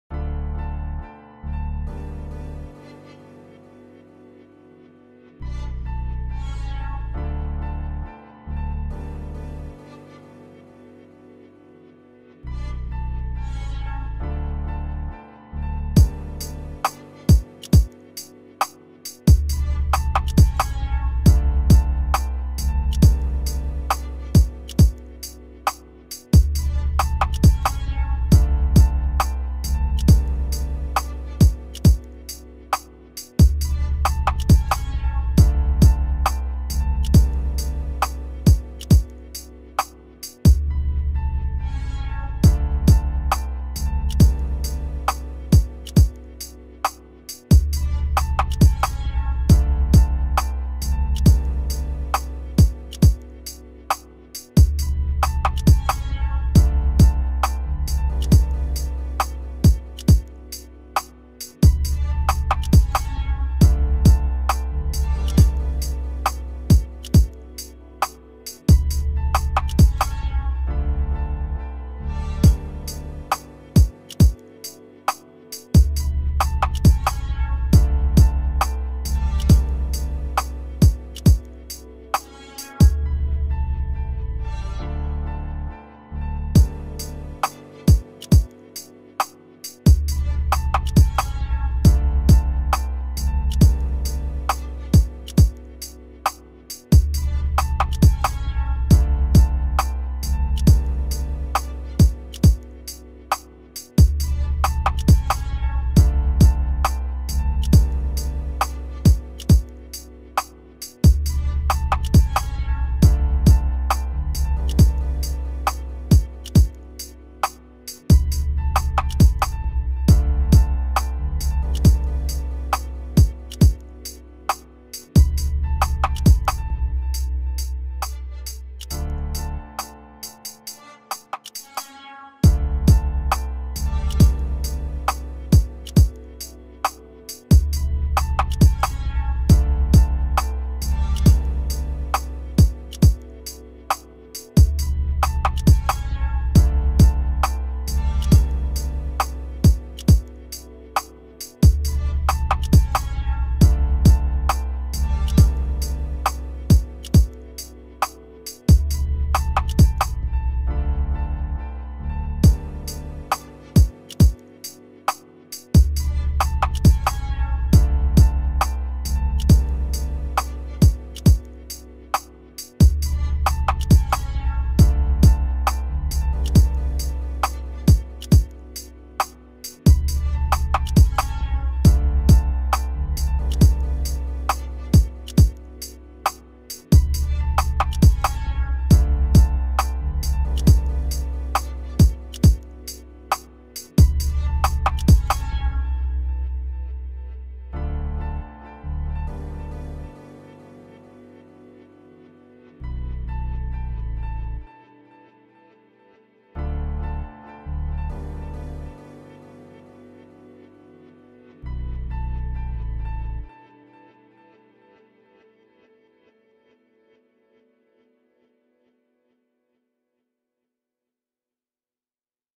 West Coast Type Beat (Offset-Prod: Blak Rose)
This beat contains west coast Dre Melodies with mix of Daz and Eminem Type Drums.

Artist: Blak Rose
Publisher: Concpts Amaru